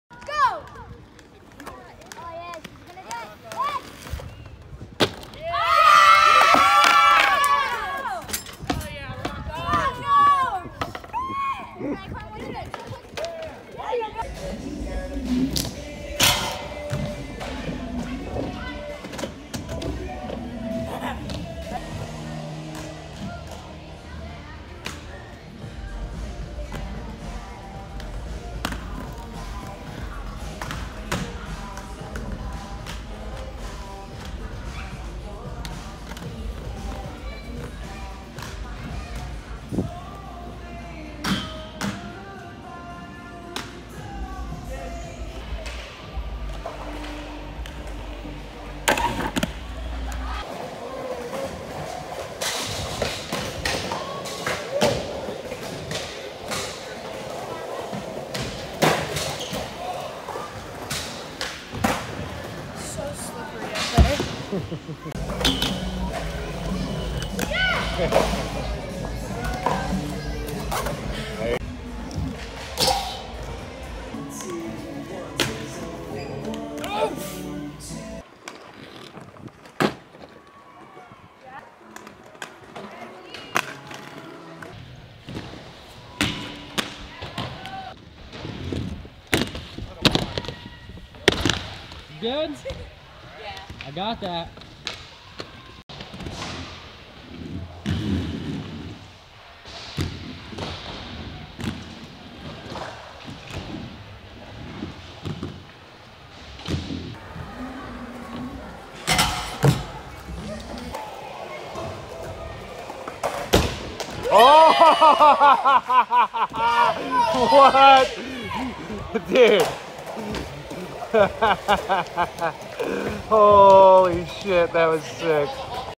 Rocco Hager - 13 Year Old Inline Skater
Rocco Hager ripping at the KTR Chandler skatepark as well as at Woodward West.